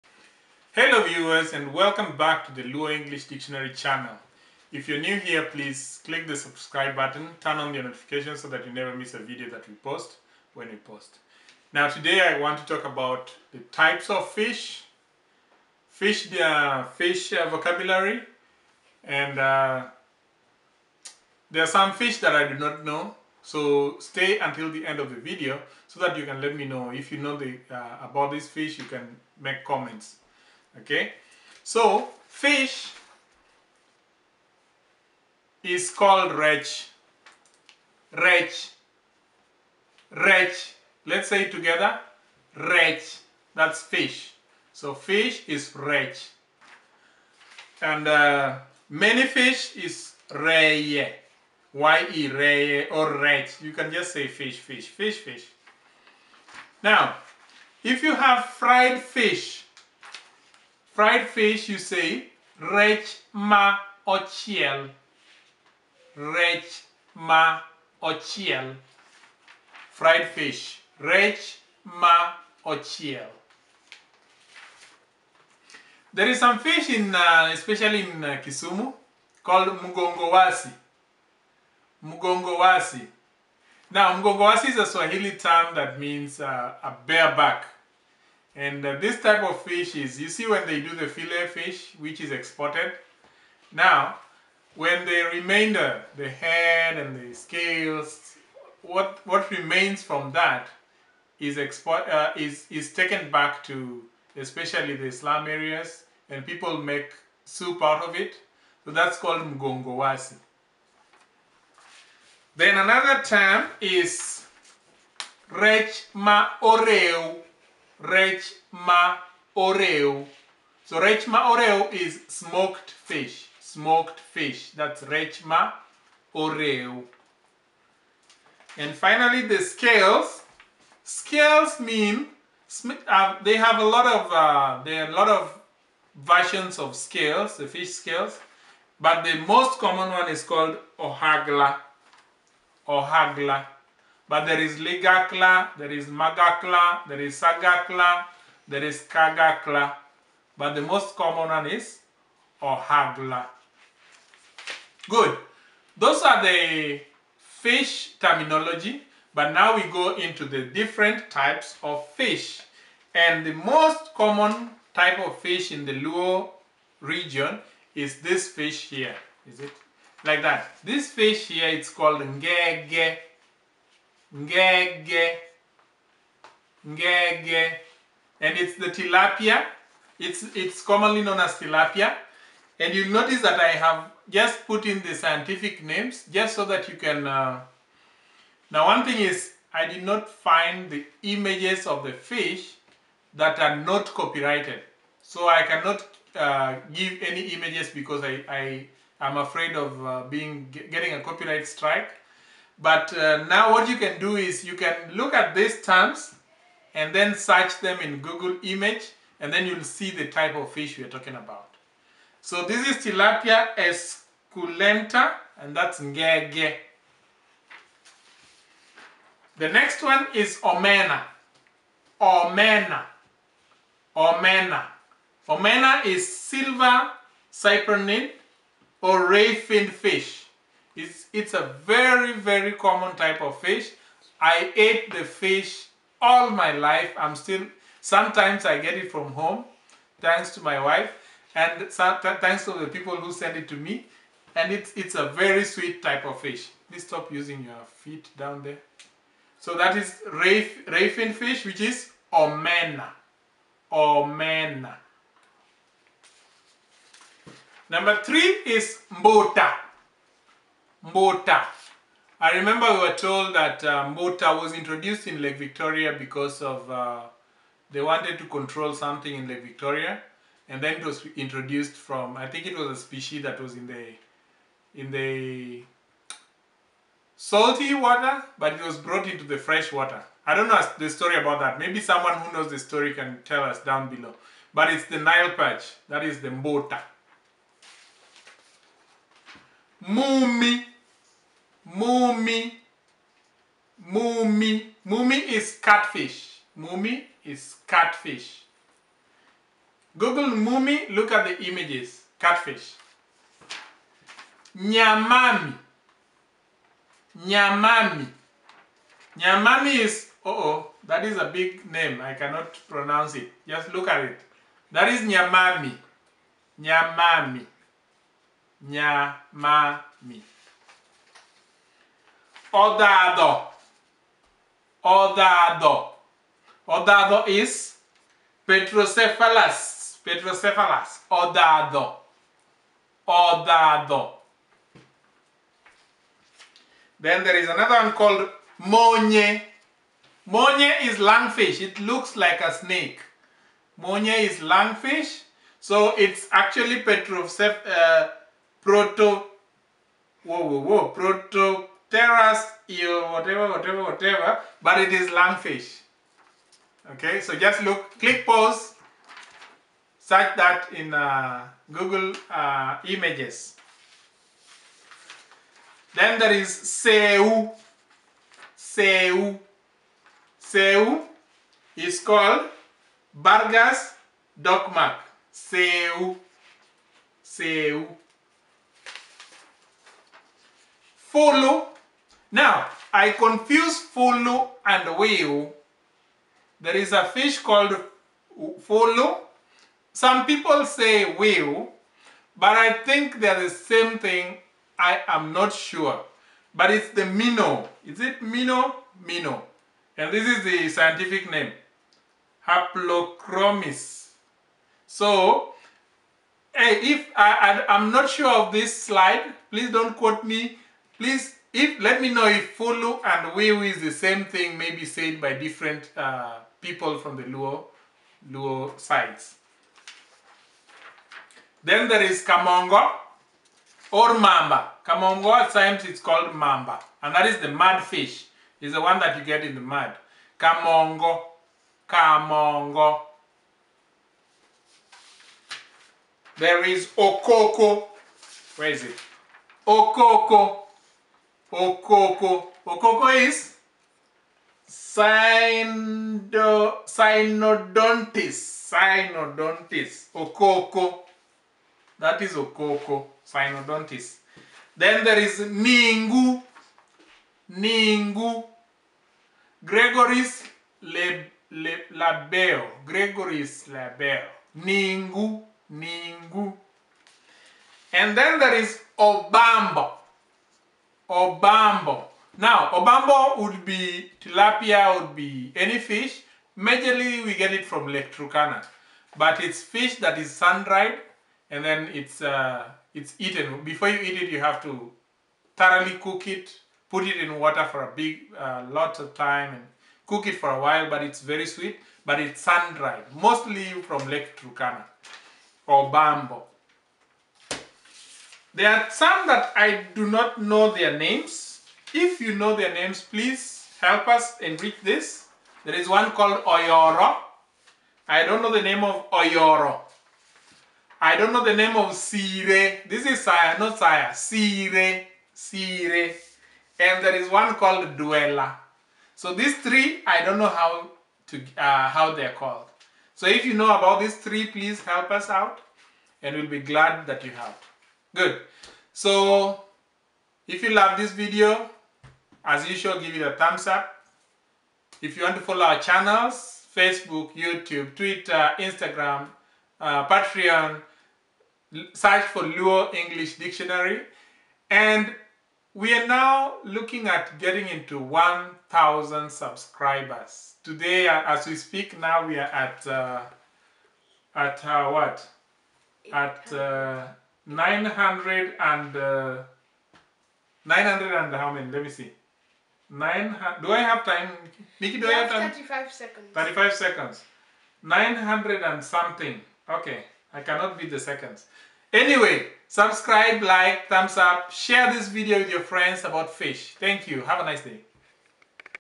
FISH LUO names of common fresh water fish EP59
Fish Luo names of fresh water fish! We inadvertently wrote the wrong spelling for "SEU" type of fish, it should be Bagrus Docmak! Sorry for that...

Let us know if you would like to send us stuff and we shall be glad to provide an address.